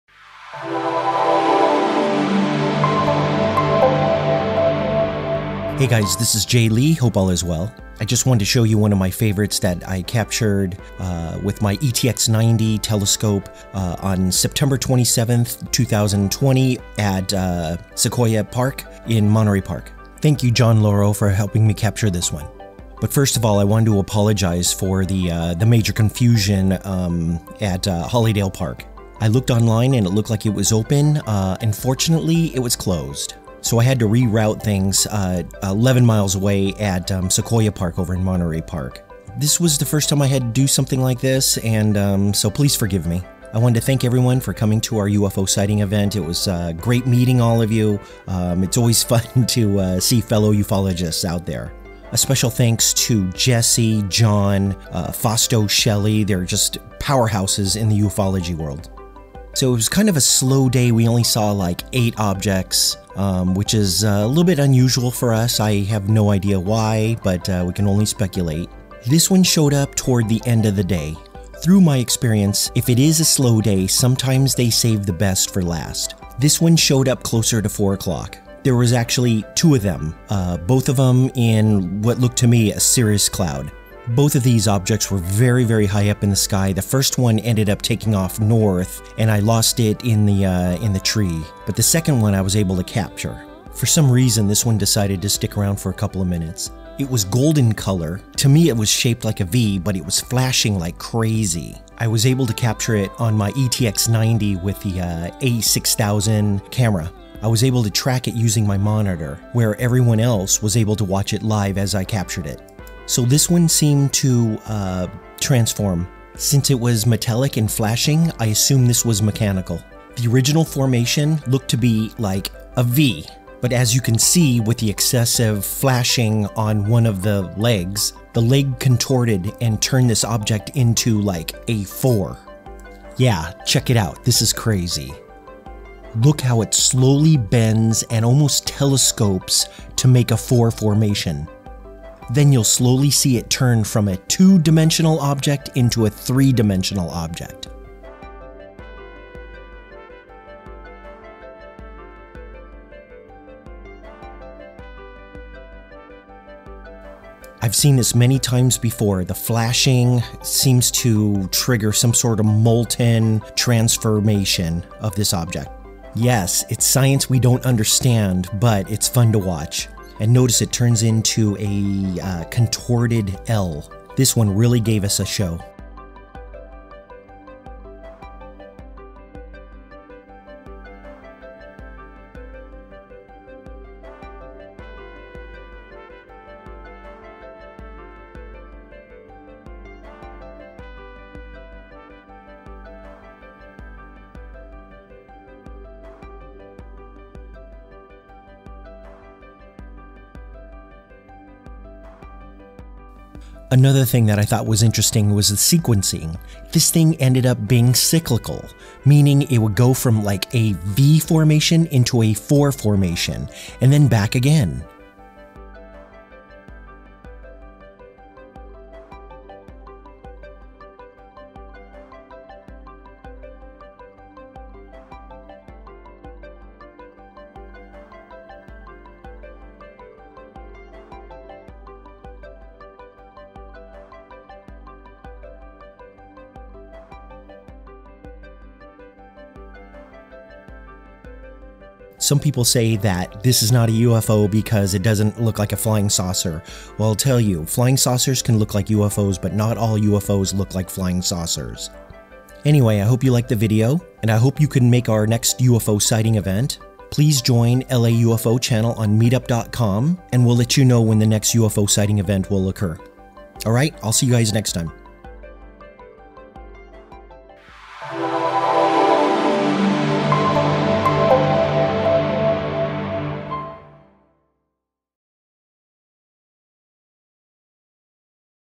Crazy Transforming UFO seen at UFO Sighting Event
It was a crazy day and we wound up going to Sequoia Park, Monterey Park, Los Angeles.  Although not many objects seen this day, one of them was really incredible.  Great people attended.  Thank you for all of your help.